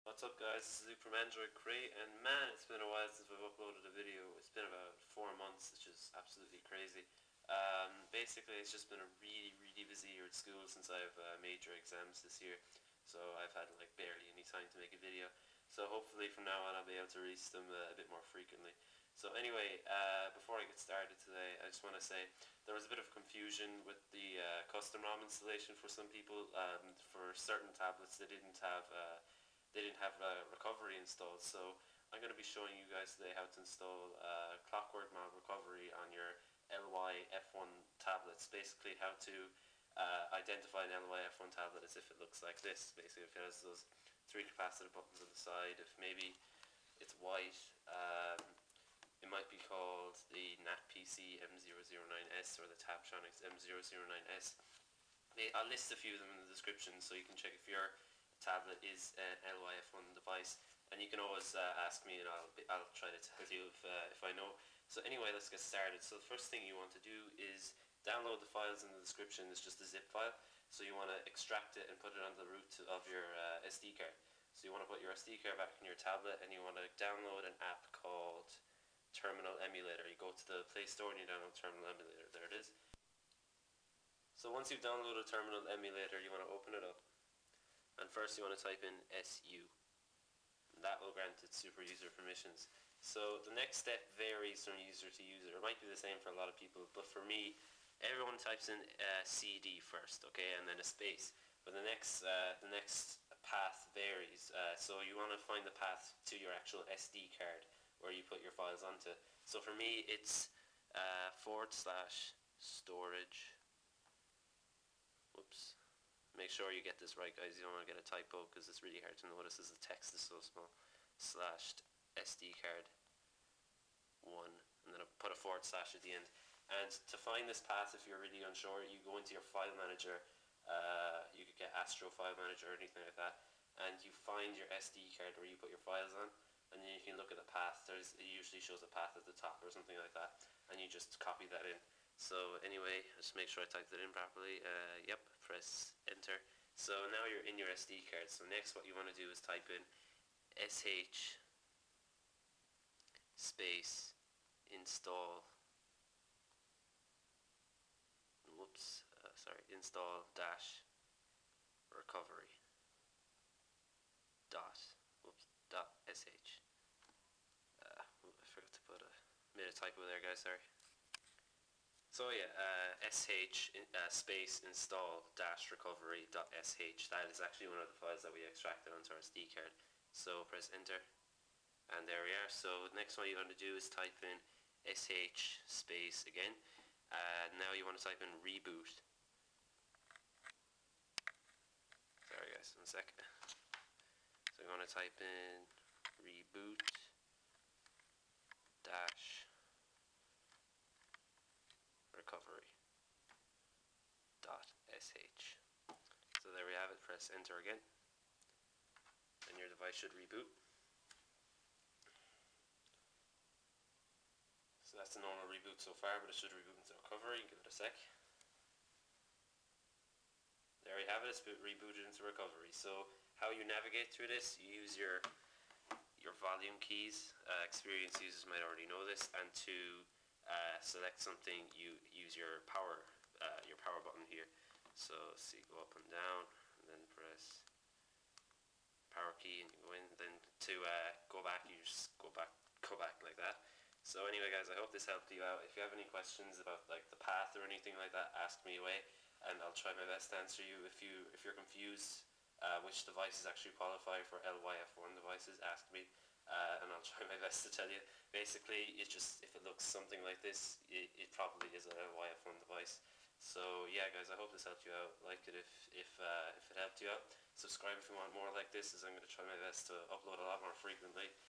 How to install CWM Recovery on LY F1 Tablets
Today I show you guys how to install ClockworkMod Recovery on your LY-F1 devices!
SD Card Path - If you do not know your SD Card path (location), go into a file manager, locate your SD Card that you put the files onto, and check the path. For example: /mnt/sdcard/  or /mnt/sdcard1/ or /storage/sdcard/  etc.

Names of some LY-F1 tablets: NATPC/TabTronics M009S, or A008 Tablet and many more. LY-F1 tablets can be characterised by the 3 capacative buttons on the side, and the speaker at the back. Ask me if you are unsure on whether or not your tablet is a LY-F1 device.